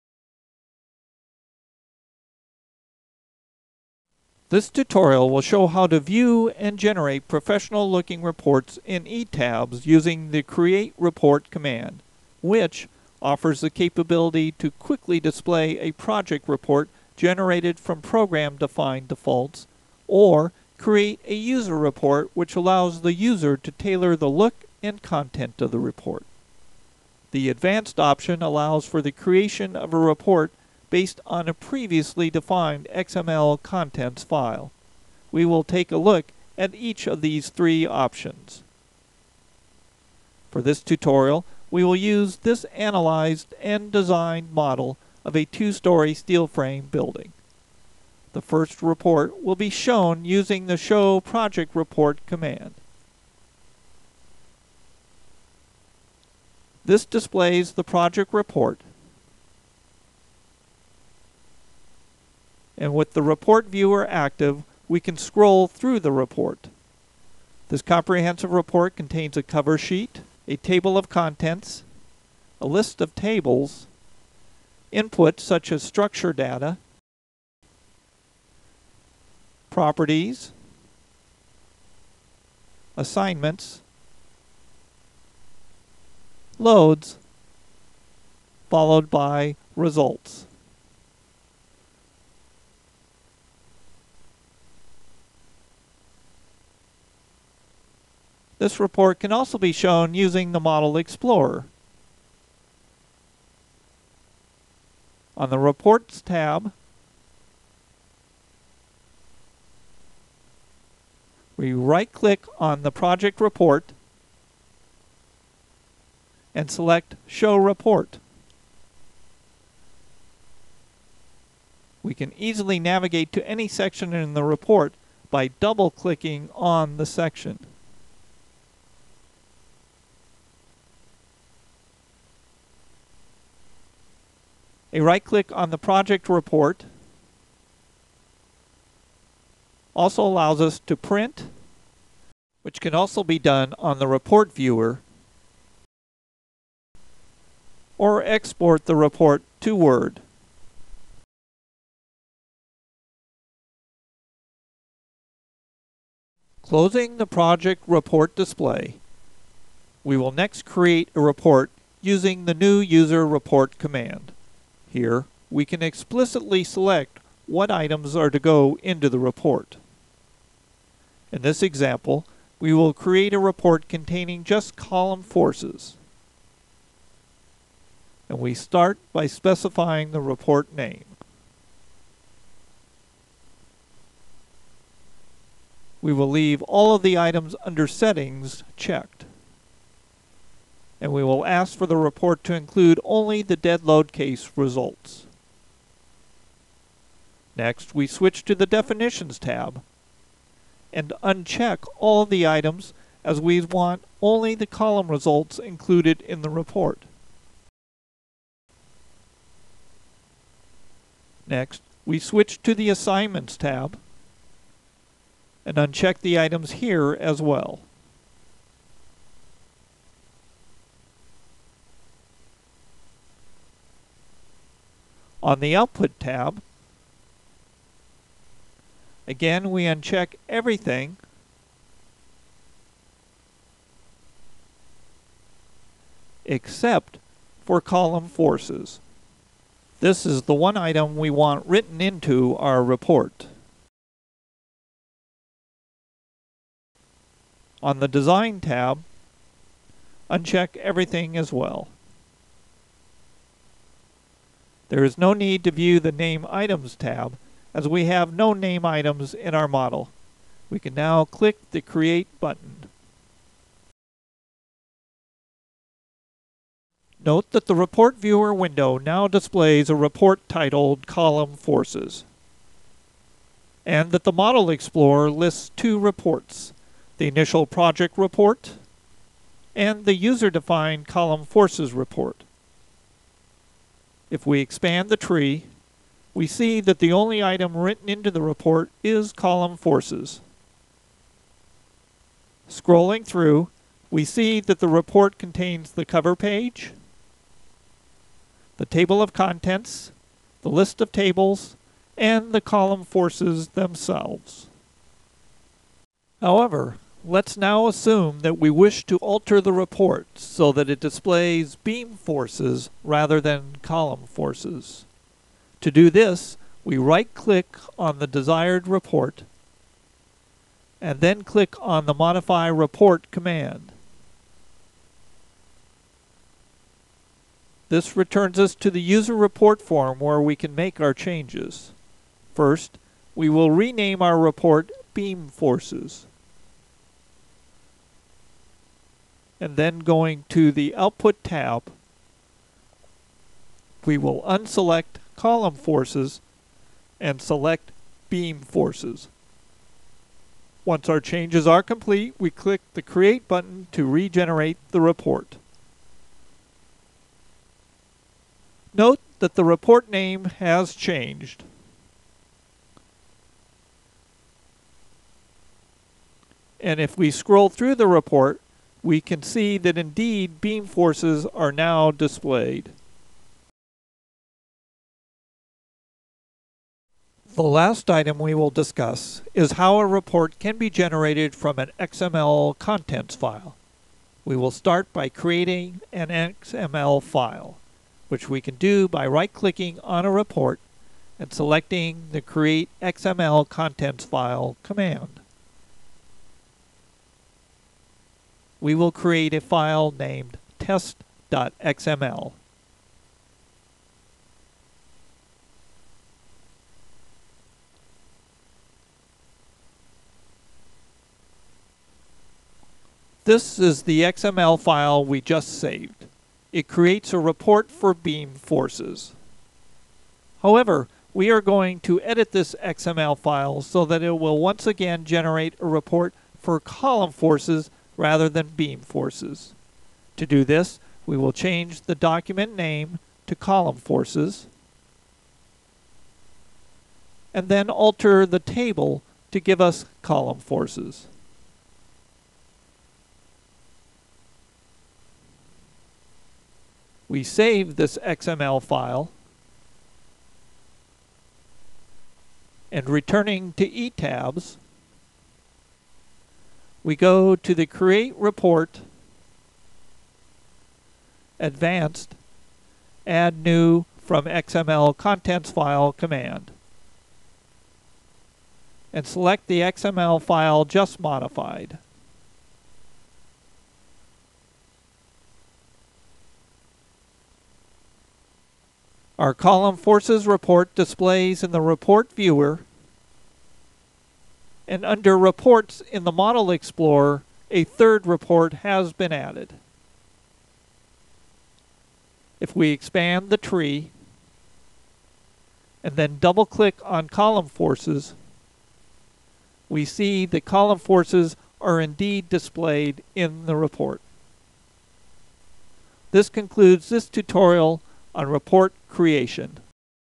ETABS - 13 Creating Reports: Watch & Learn
Learn about the ETABS 3D finite element based building analysis and design program and how to generate professional looking reports that describe the input details and output results.